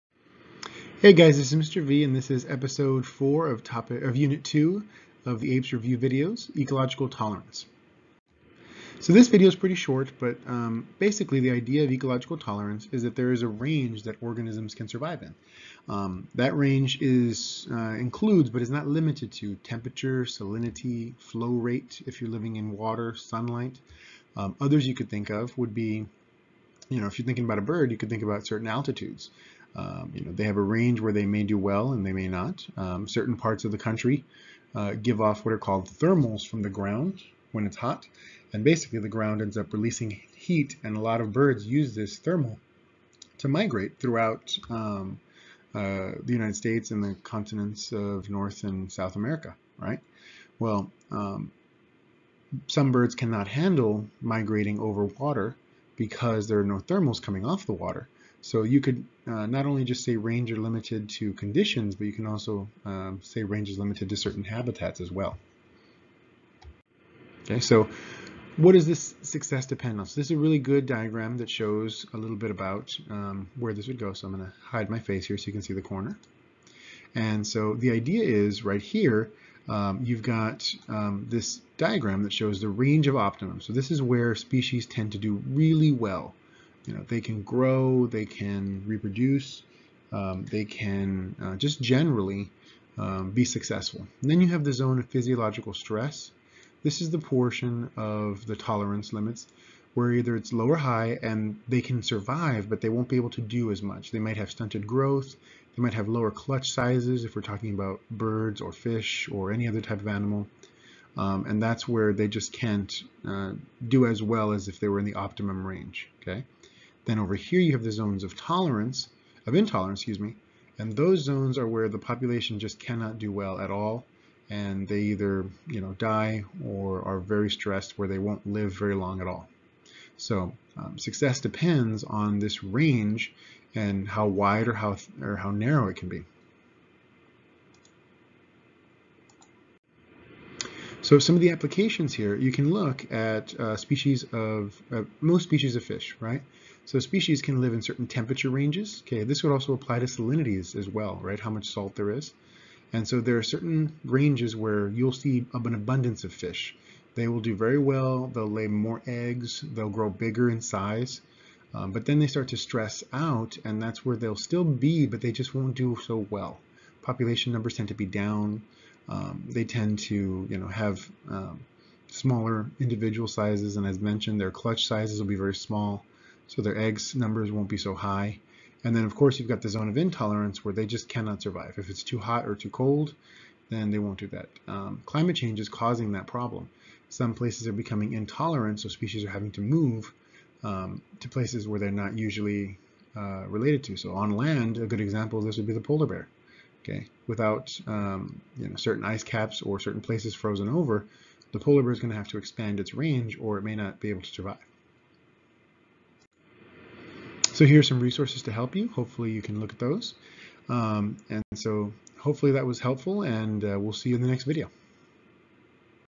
APES Topic 2.4, Ecological Tolerance
This is Unit 2, topic 4, Ecological Tolerance, from the AP Environmental Science CED